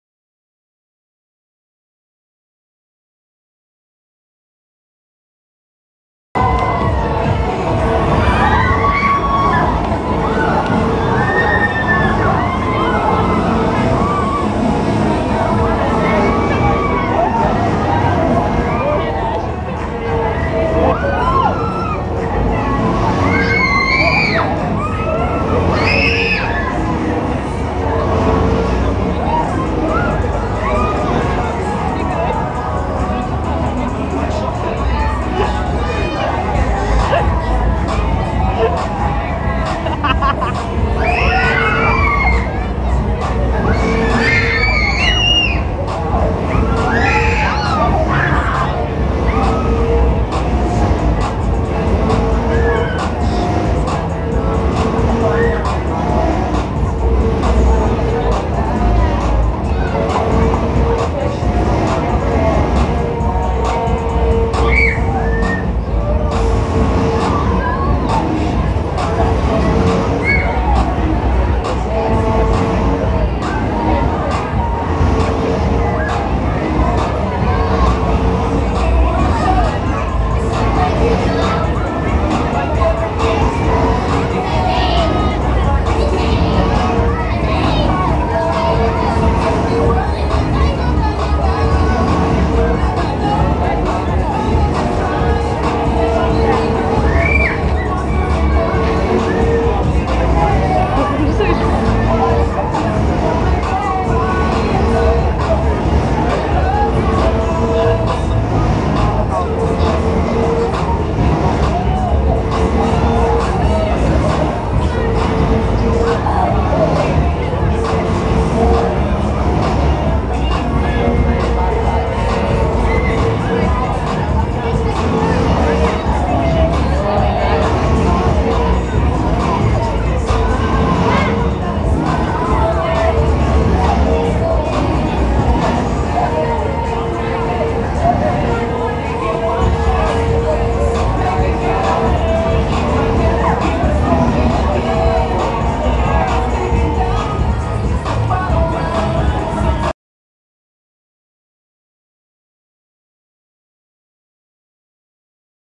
Me on DANCE FEVER
ride was fun but cut short due to the little girl one seat over from me being scared to death.

ride was much fun for me due to a certain piercing getting action everytime the ride swung up to the side.